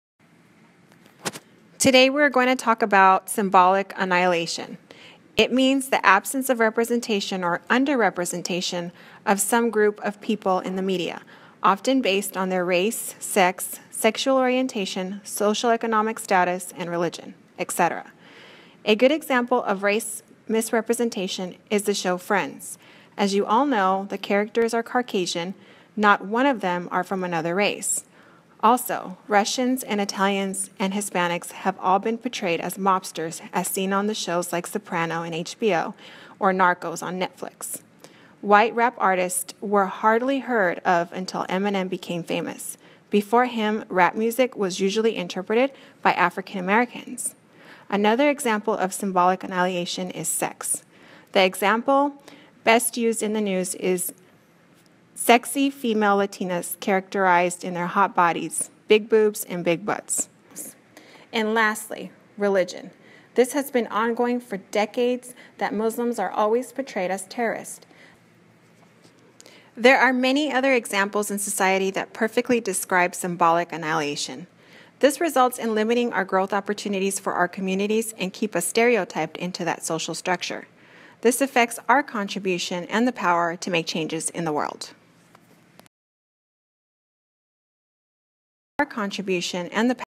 SYMBOLIC ANNIHILATION - MEDIA 1
This video explains what is Symbolic Annihilation.